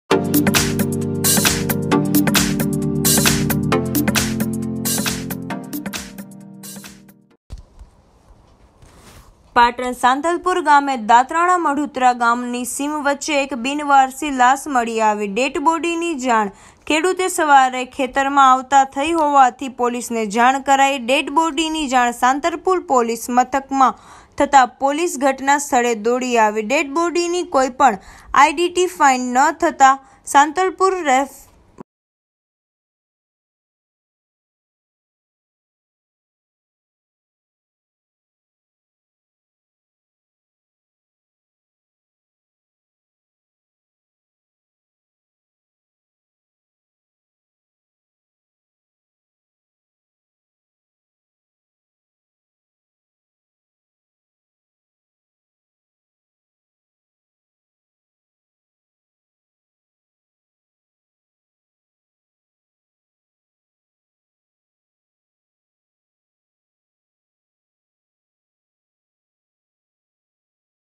PATAN સાંતલપુર ગામે દાત્રાણા મઢુત્રા ગામની સીમ વચ્ચે એક બિન વરસી લાસ મળી આવી....
પાટણ સાંતલપુર ગામે દાત્રાણા મઢુત્રા ગામની સીમ વચ્ચે એક બિન વરસી લાસ મળી આવી.

ડેટ બોડી ની જાન ખેડૂત સવારે ખેતરે આવતા ખેડૂત ને થતાં પોલીસ ને જાણ કરાઈ.

ડેટ બોડી ની જાણ સાંતલપુર પોલીસ મથક મા થતા પોલીસ ઘટના સ્થળે દોડી આવી.

ડેટ બોડી ની કાંઈ પણ આઇ ડી ટી ફાઈ ન થતાં સાંતલપુર રેફરલ હોસ્પિટલ માં લવાઈ.

પોલીસ તપાસ માં ડેટ બોડી કોની છે તેની કાંઈ હાલ ઓળખ થયેલ નથી.

અગાઉ   એક મહિના માં આ વિસ્તાર માં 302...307 જેવા ગુના નોંધાયેલા છે...

જયારે આજ એક બિનવરસી ડેટ બોડી મળતાં સાંતલપુર વિસ્તાર માં દિવસે ને દિવસે વધતાં ગુનાહિત પ્રવૃત્તિ ને લઈને લોકો માં ભય અને ડર નો માહોલ...

પાટણ બ્યુરોચીફ..
ઈન્દુભા વાઘેલા..
સાંતલપુર